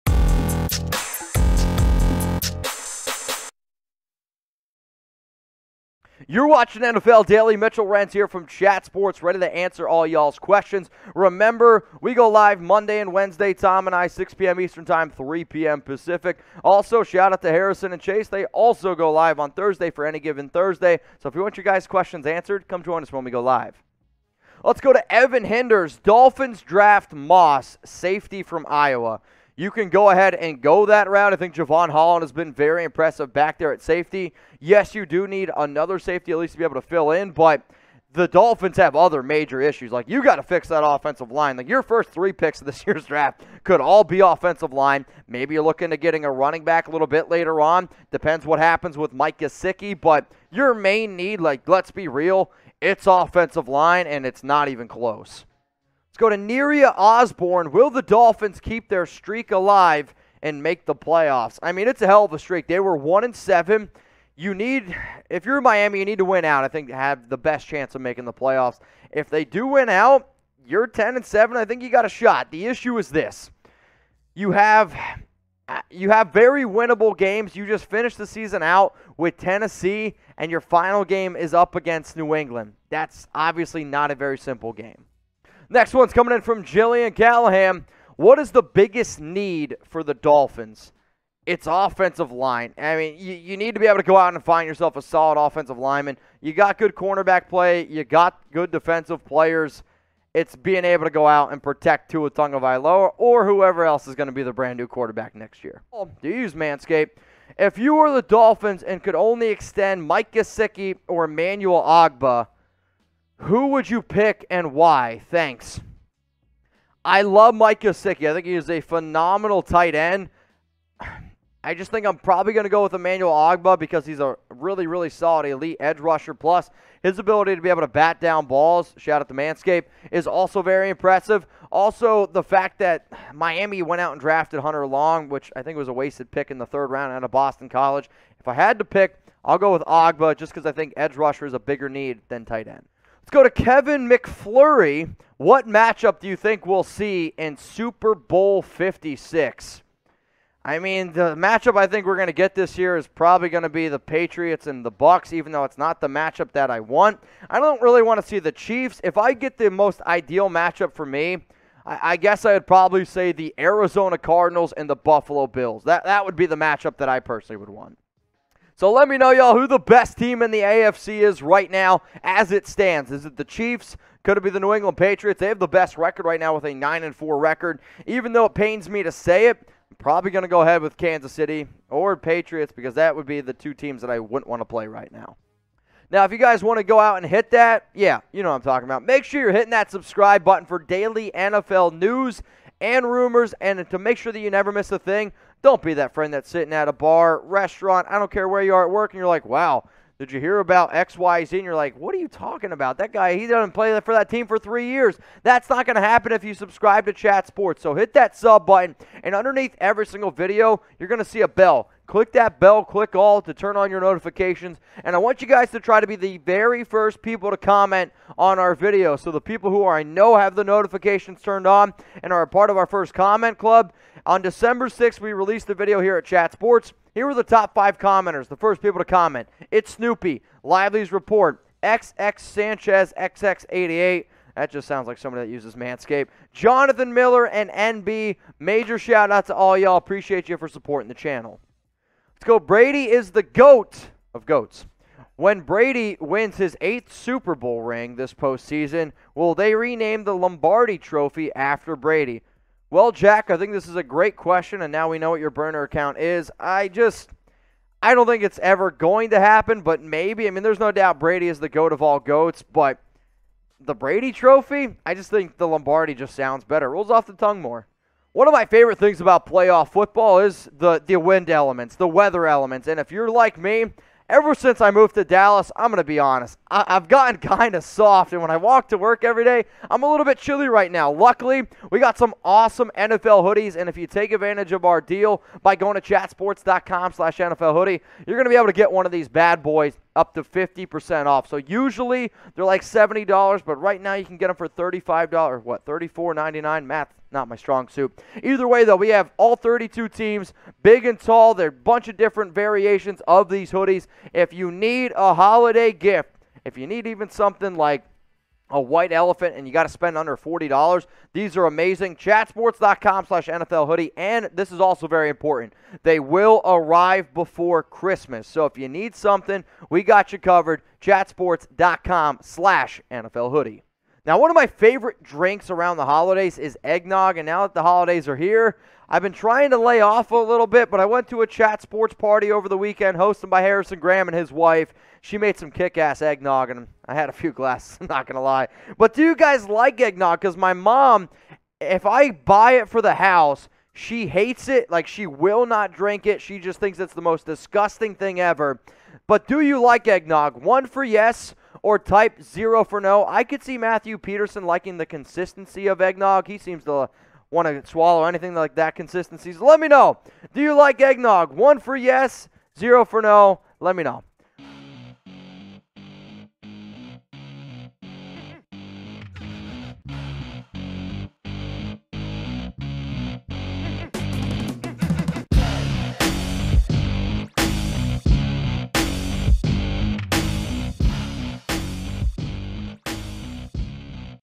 Dolphins Rumors Mailbag: 2022 NFL Draft Targets + Mike Gesicki & Emmanuel Ogbah Contract Extensions
Miami Dolphins rumors today are all about the NFL Playoffs. Can Tua Tagovailoa and Miami keep their winning streak going and find themselves inside the AFC Playoff Picture at the end of the 2021 NFL Season? Dolphins Rumors also discuss tight end Mike Gesicki and DE Emmanuel Ogbah, who could be in line for contract extensions this NFL offseason. The Dolphins have a bye in NFL Week 14 before taking on the New York Jets in NFL Week 15. Dolphins rumors also explore the possibilities of NFL free agency and how the Dolphins can improve their team.

Here are the questions from today’s video:
- Should the Dolphins draft Riley Moss in the 2022 NFL Draft?
- Can the Dolphins keep their winning streak alive and make the NFL Playoffs
- What is the biggest need for the Dolphins heading into the 2022 NFL Season?
- Mike Gesicki & Emmanuel Ogbah contract extensions
- Super Bowl 56 matchup prediction?
-  Will they rename the Lombardi Trophy after Tom Brady?

Want the Dolphins hoodie that Mitchell Renz mentioned on today’s show? Here ya go!

Dolphins Playoff Chances After 5 Straight Wins + NFL Playoff Picture & Who To Root For In Week 14